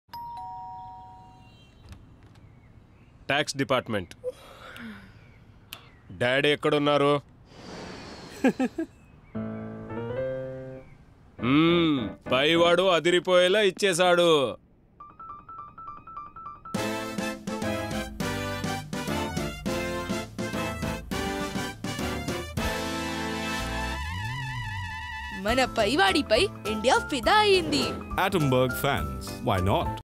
Atomberg TVC I Tax Department comes knocking | Telugu
ATOM and BERG are back.

This time with an ‘Ooparwala’ that will leave India speechless.

And…yes this ‘Ooparwala’ can be controlled by a remote.

Atomberg - 5 star rated BLDC fans